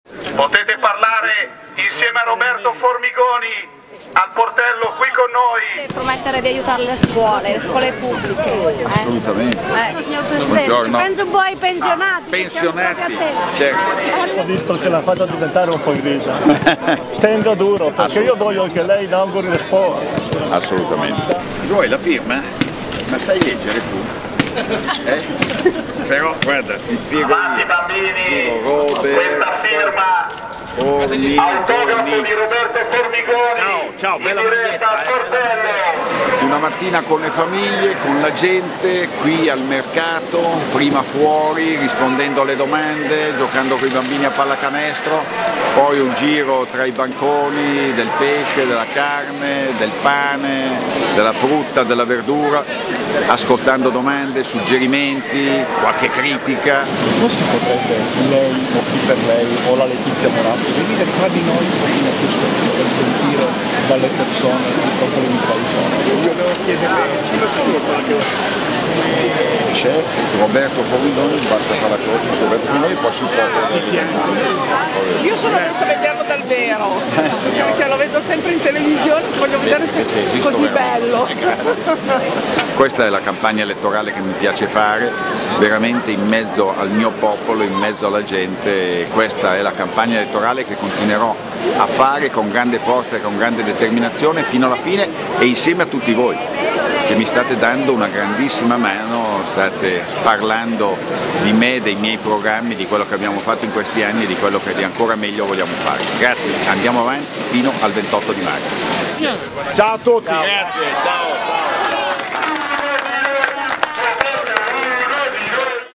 Formigoni con le famiglie al Portello
Il presidente della Regione Lombardia Roberto Formigoni incontra le famiglie al centro commerciale di piazza Portello a Milano